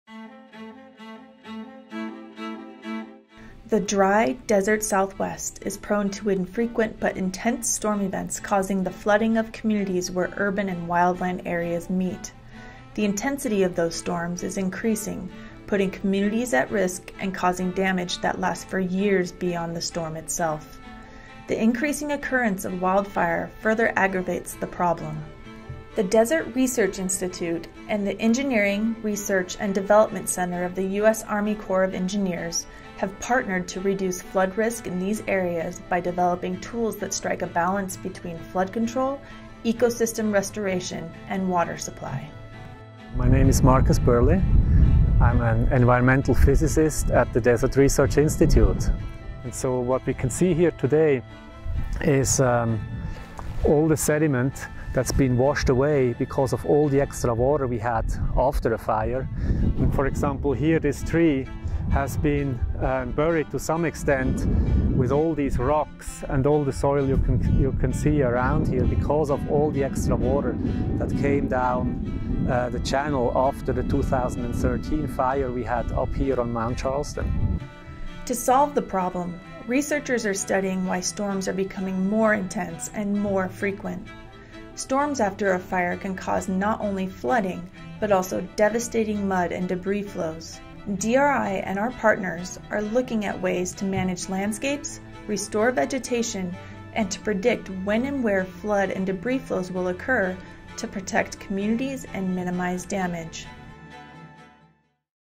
Urban Flood Research Program
The Desert Research Institute and the Engineering, Research and Development Center of the US Army Corps of Engineers have partnered to reduce flood risk in the desert southwest, which is prone to infrequent but intense storms, by developing tools that strike a balance between flood control, ecosystem restoration, and water supply.

To solve the problem, researchers are studying why storms are becoming more intense and more frequent.  Storms after a fire can cause not only flooding but also devastating mud and debris flows.   DRI and our partners are looking at ways to manage landscapes, restore vegetation, and to predict when and where floods and debris flows will occur to protect communities and minimize damage.